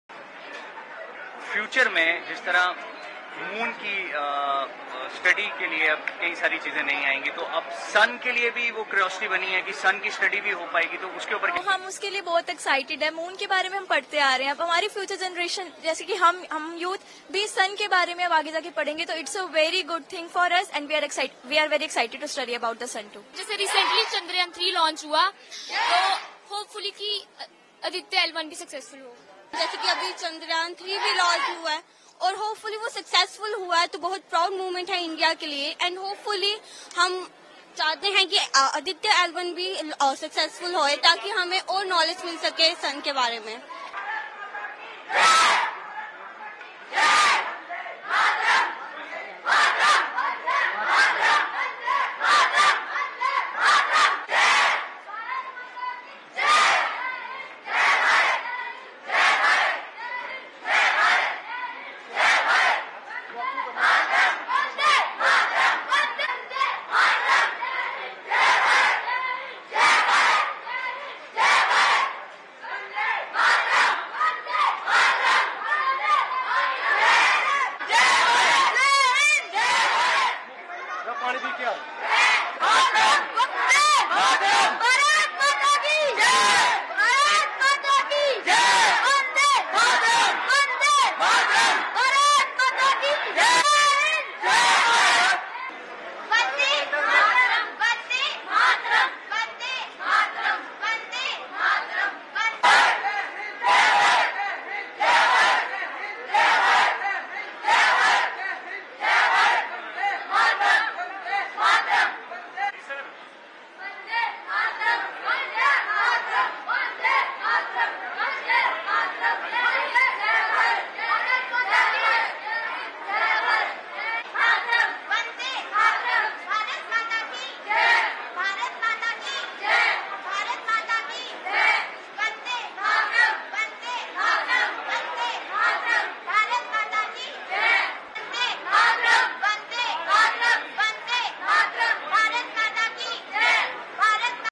Aditya L1 Latest News: आदित्य L1 Mission को लेकर इन बच्चों की बात दिल खुश कर देगी! | N18V
ISRO Aditya L1 Mission Launch Live Updates: चांद के दक्षिणी ध्रुव पर चंद्रयान 3 की ऐतिहासिक लैंडिंग के बाद भारतीय अंतरिक्ष अनुसंधान संगठन  (इसरो) एक बार फिर इतिहास रचने की दहलीज पर है.  अब देश के साथ-साथ विश्व देश की निगाहें ISRO के सूर्य मिशन यानी Aditya-L1 पर टिकी हैं. श्रीहरिकोटा के लॉन्चिंग सेंटर से ISRO के सूर्य मिशन आदित्य-L1 मिशन को आज 11.50 बजे लॉन्च कर दिया गया. आदित्य एल-1 अंतरिक्ष यान को पृथ्वी और सूर्य के बीच की एक फीसदी दूरी तय करके L-1 पॉइंट पर पहुंचा देगा.